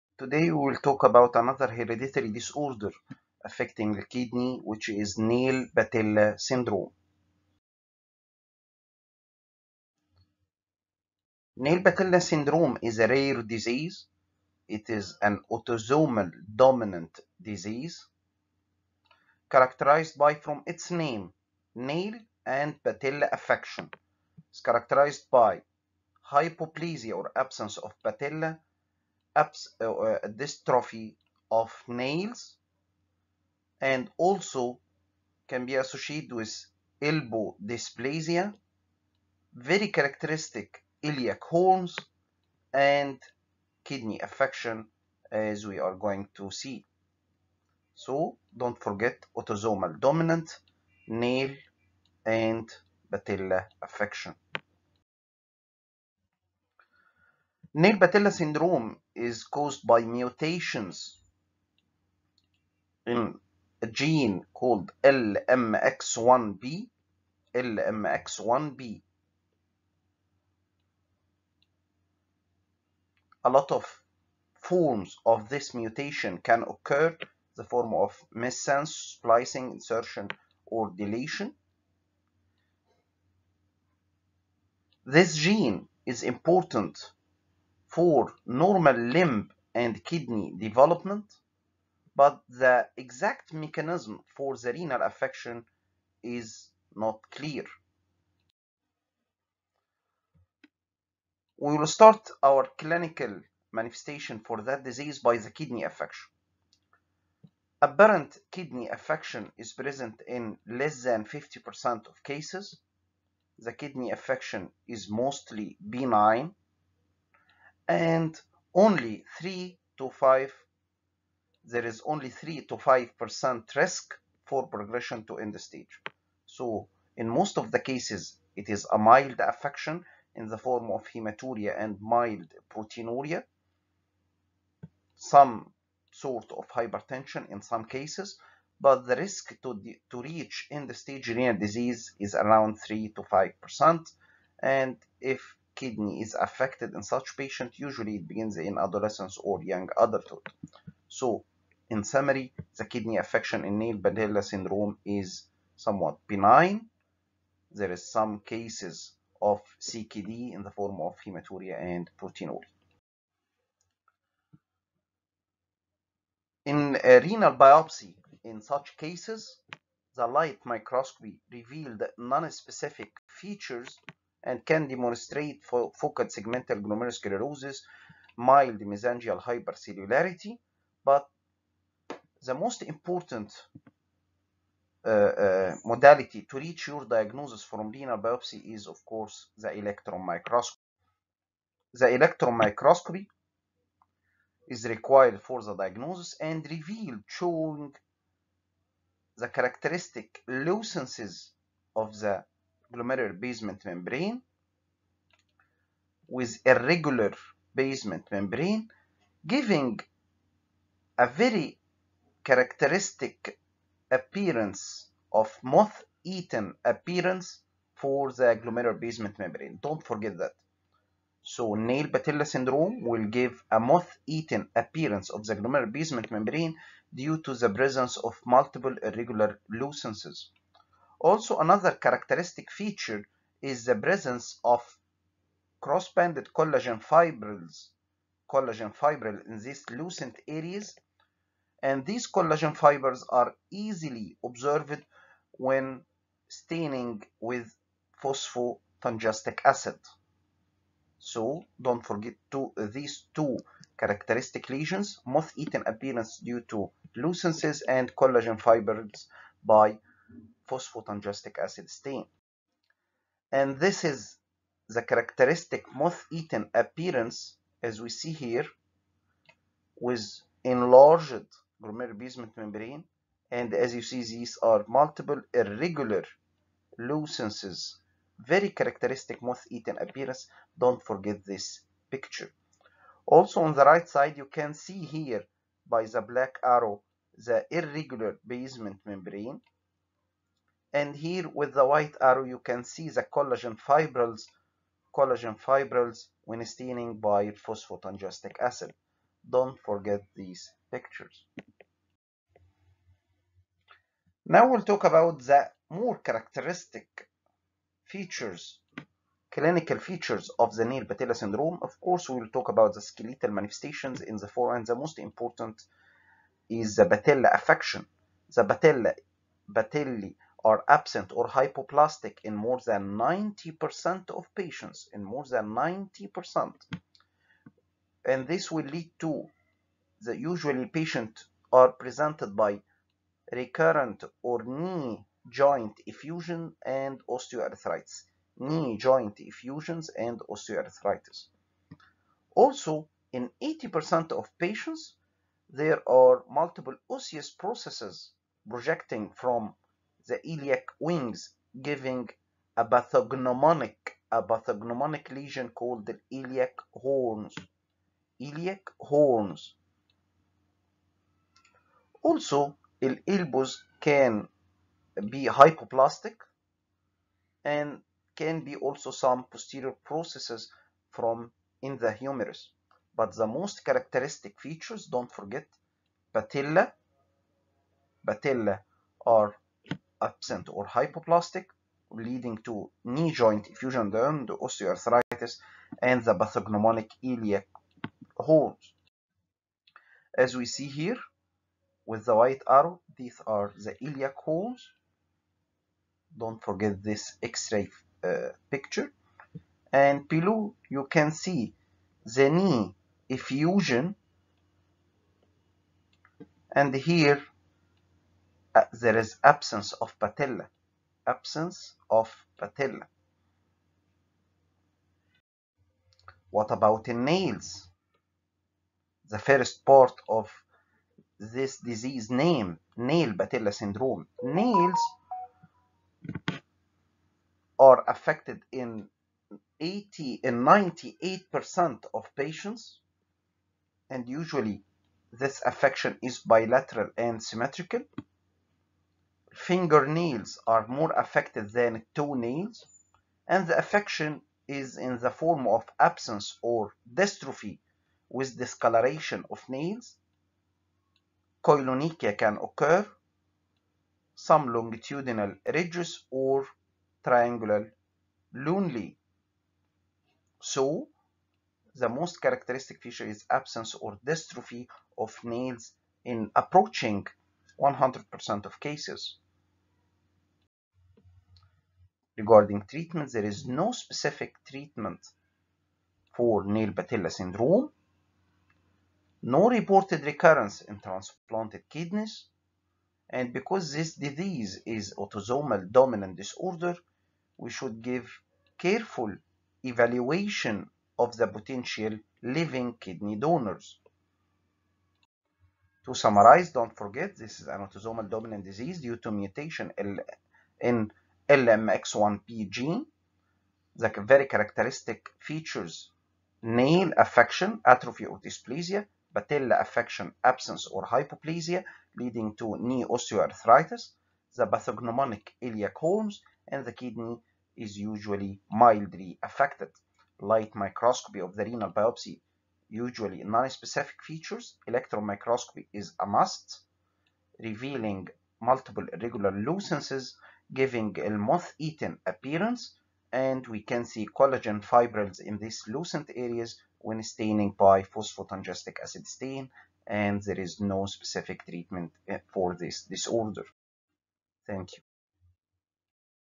Nail patella syndrome
nail patella syndrome is one of the hereditary disorders affecting kidney